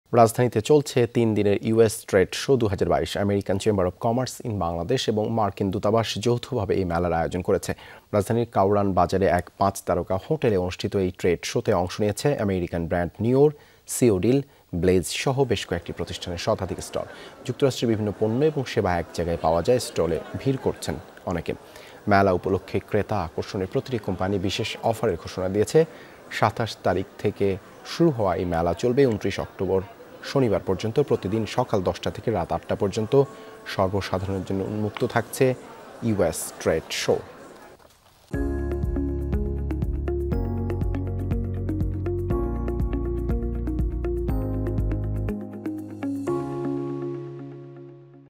ঢাকায় চলছে তিন দিনের ইউএস ট্রেড শো ২০২২ | US TRADE SHOW
রাজধানীতে চলছে, তিন দিনের ইউএস ট্রেড শো ২০২২। আমেরিকান চেম্বার অব কমার্স ইন বাংলাদেশ এবং মার্কিন দূতাবাস যৌথভাবে এ মেলার আয়োজন করেছে।